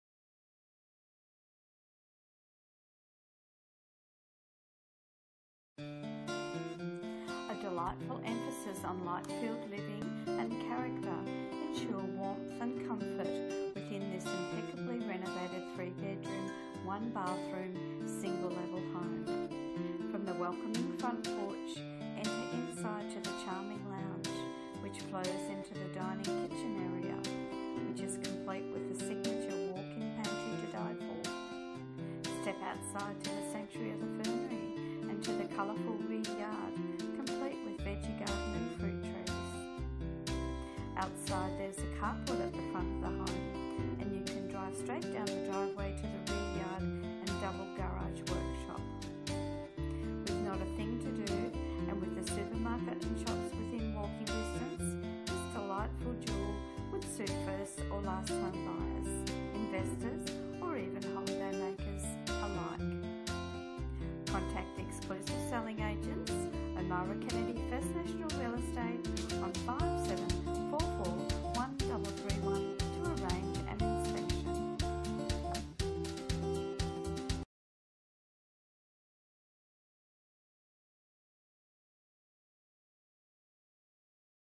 86 Sharp Street Yarrawonga, Victoria
IMPECCABLE SUNLIT SANCTUARY - A delightful emphasis on light-filled living and character ensure warmth and comfort within this impeccably renovated 3 Bedroom, 1 bathroom single level home. From the welcoming front porch, enter inside to the charming lounge which flows into the dining/kitchen area (complete with a signature walk in pantry to die for).
Step outside to the sanctuary of the fernery and to the colourful rear yard complete with vegie garden and fruit trees. Outside there is a carport at the front of the home and you can drive straight down the driveway to the rear yard and double garage/workshop.
With not a thing to do and with the Supermarket & shops within walking distance, this delightful jewel would suit first or last home buyers, investors or even holiday makers alike.